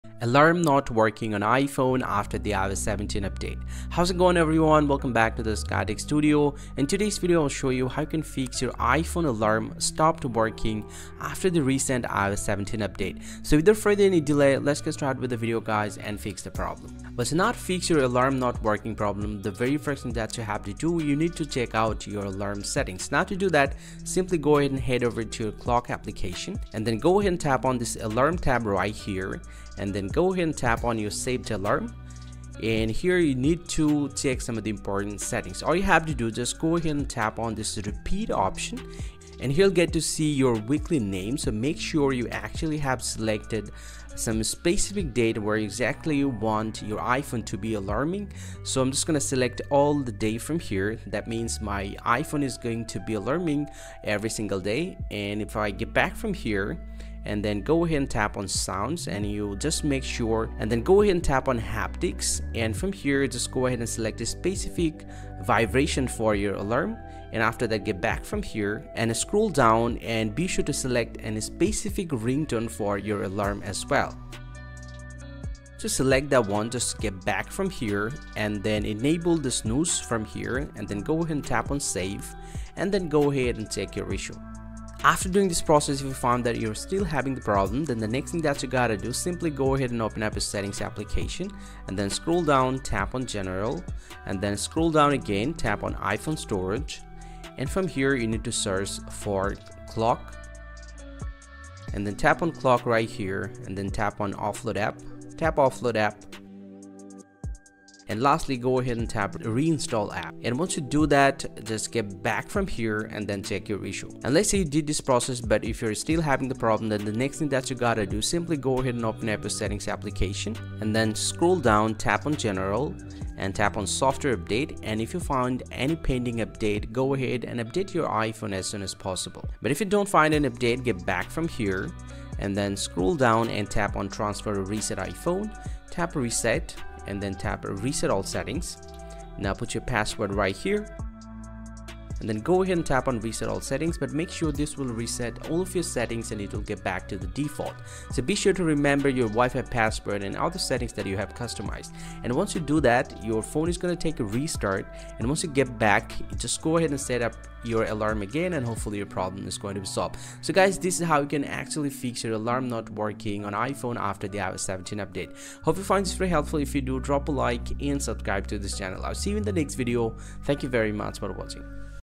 How To Fix iPhone Alarm Not Working After iOS 17 Update | iOS 17 Alarm Glitch
iOS 17 alarm glitch
iPhone alarm sound not working
how to fix iPhone alarm not working on iOS 17?

Is your iPhone alarm not working after iOS  17 update? If you found iPhone alarm not working on iOS  17 and you want to fix fix iPhone alarm then keep watching the video till the end.
Many iPhone users have been asking why does my iPhone alarm have no sound iOS 17? and How do I turn on my alarm on iOS 17?

00:01- Video Starting
00:16- Solution 1: Check your Alarm Settings
01:24- Solution 2: Offload the Clock App
01:54- Solution 3: Check for update
02:09- Solution 4: Reset all settings
02:45- Solution ending